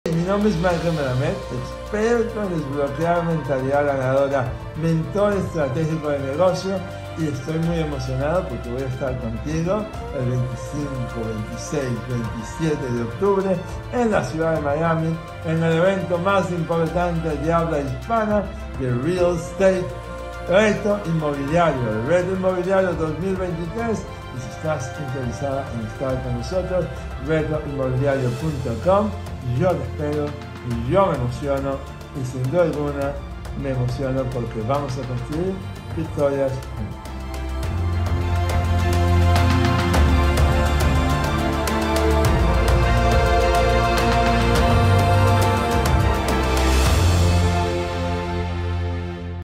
Maickel Melamed | Retiro Inmobiliario Octubre 2023
Maickel Melamed será speaker en nuestro próxima edición del Retiro Inmobiliario los próximos 25, 26 y 27 de Octubre. No te lo puedes perder.